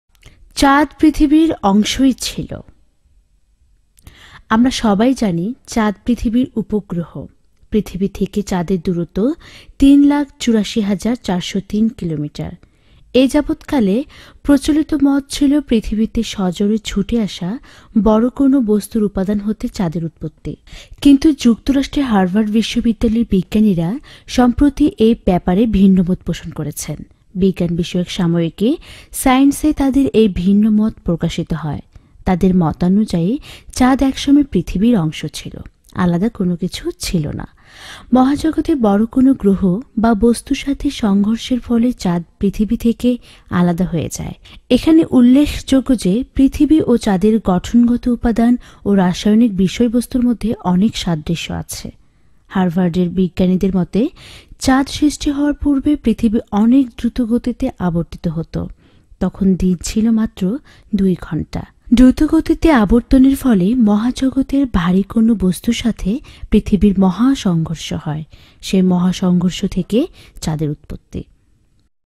Moon was a part of the world. চাঁদ একসময়ে পৃথিবীর অংশই ছিল।
➤Video Description

We think moon and world was separate from ancient time.But scientist have discover moon and world were not separate, they were part of any other.

➤  People & Blogs   Video

* Copyright Alert *
Attention Please!!!

   (Thanks & Regards)
Absar Uddin Faraege
Creator-Faraege Media.